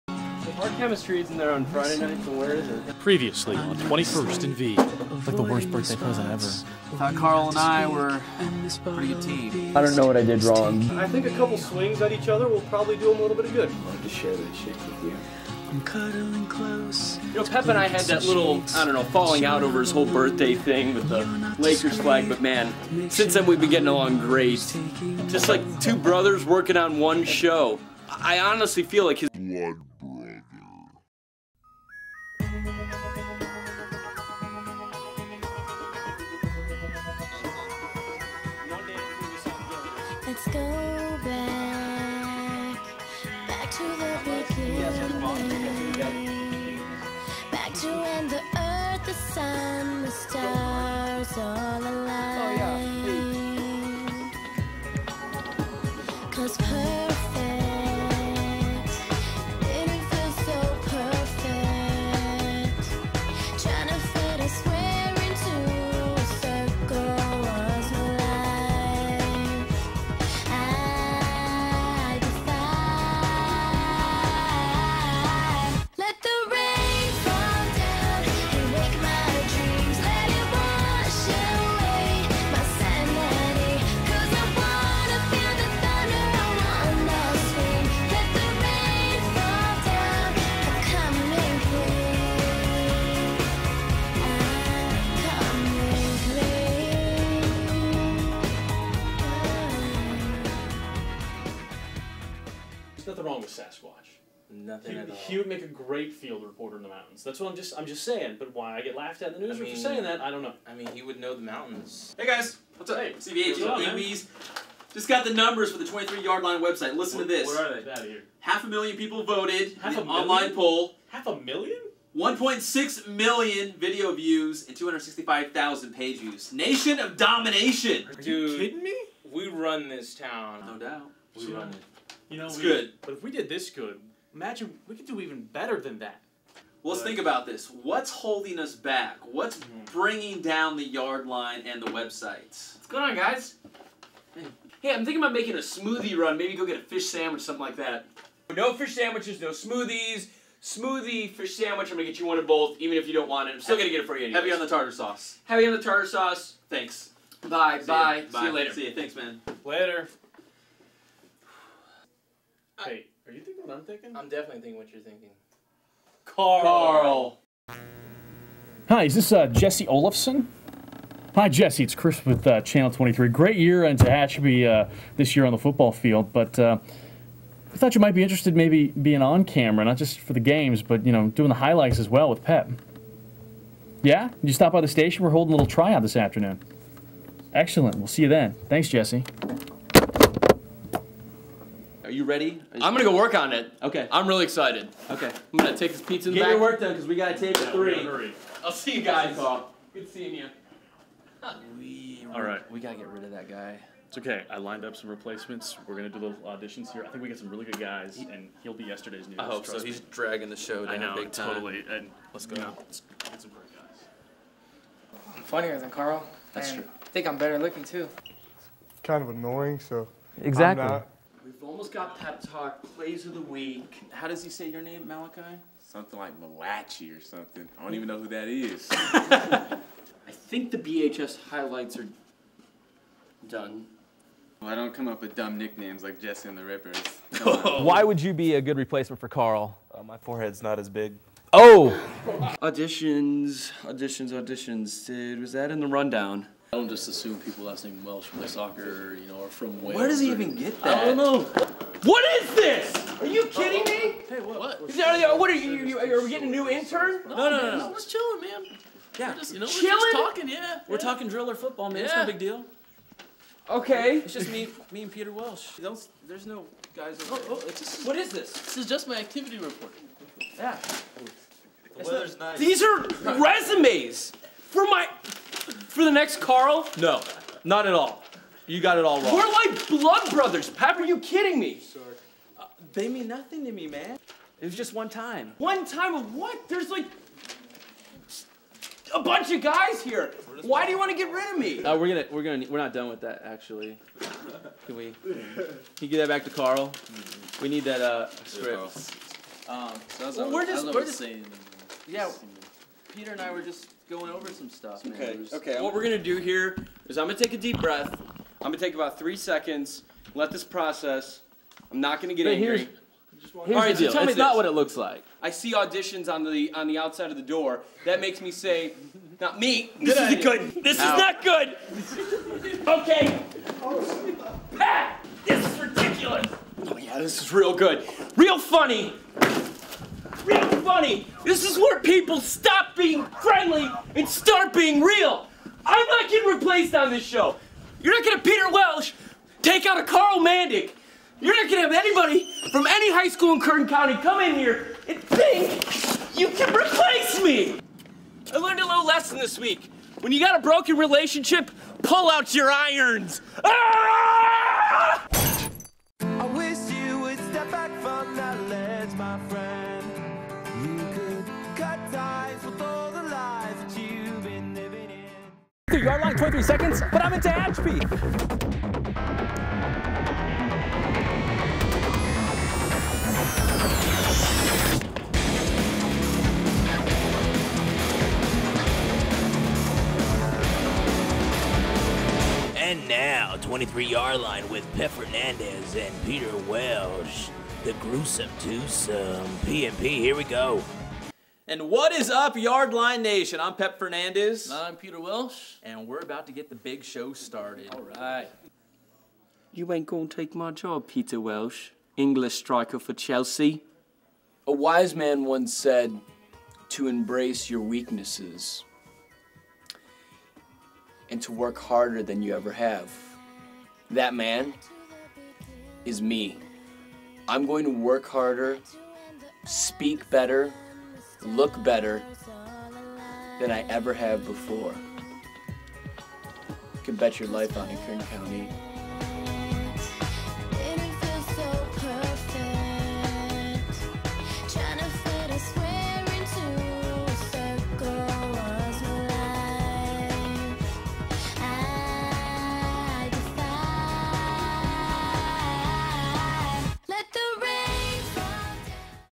21st and V: Blood Brothers
Ladies and Gents. THE 23 Yardline gives you the second installment of 21st and V!
Enjoy as Pep looks to replace Karl!